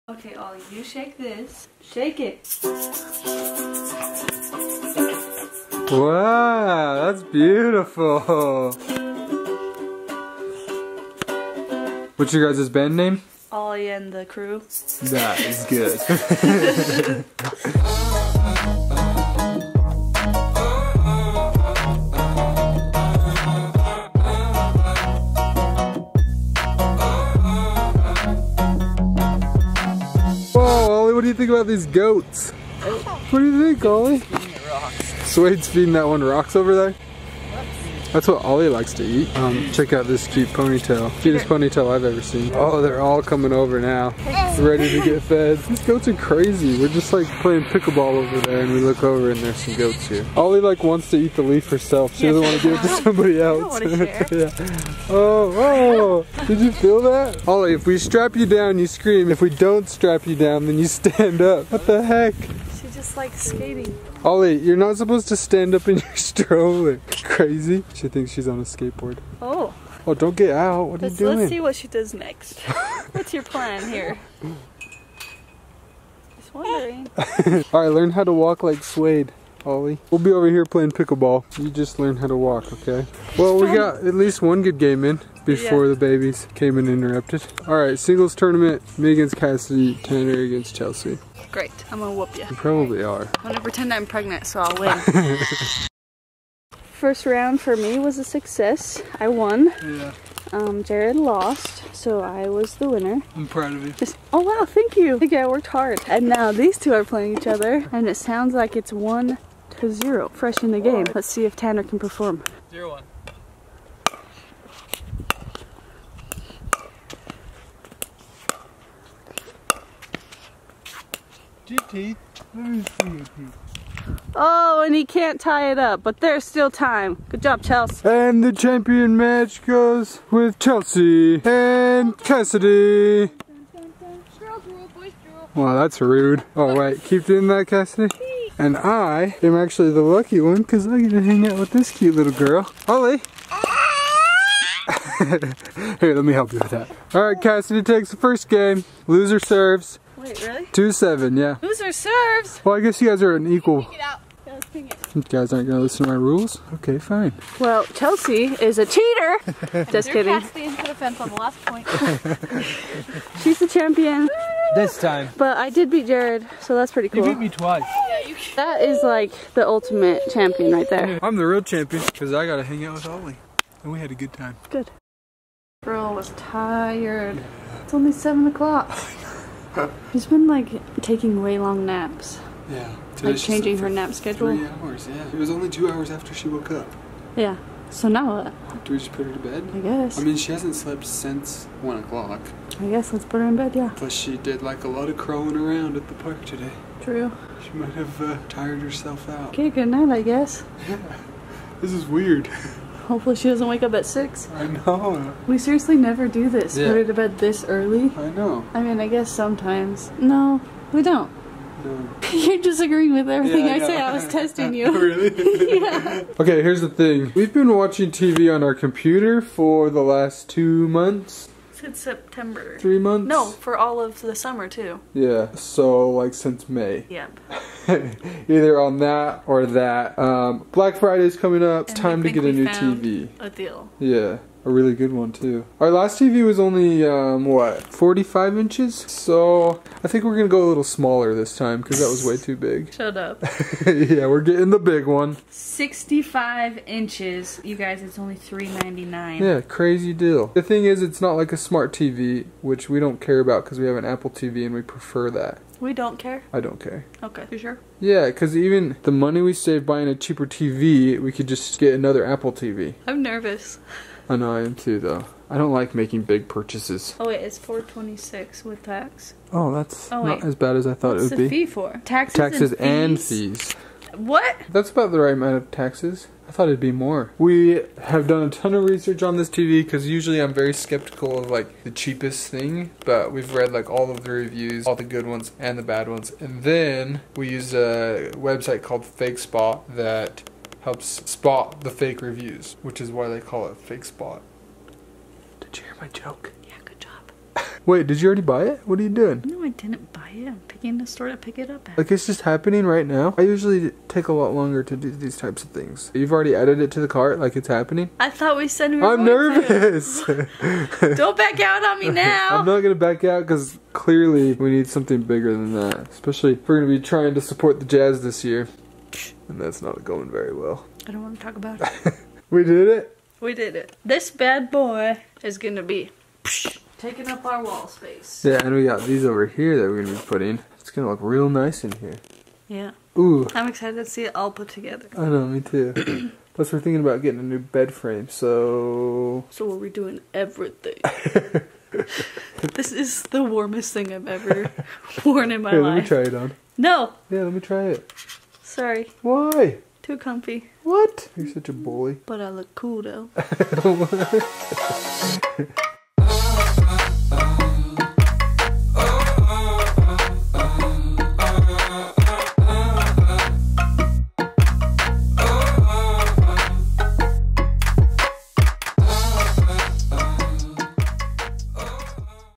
We Never Do This...
It just ended up being a very weird day.

We really never do this and it was so weird how Ollie our little 1 year old was acting. When this happens we don't really know how to react but we know that it is going to be an interesting night.

Secret Emoji:😎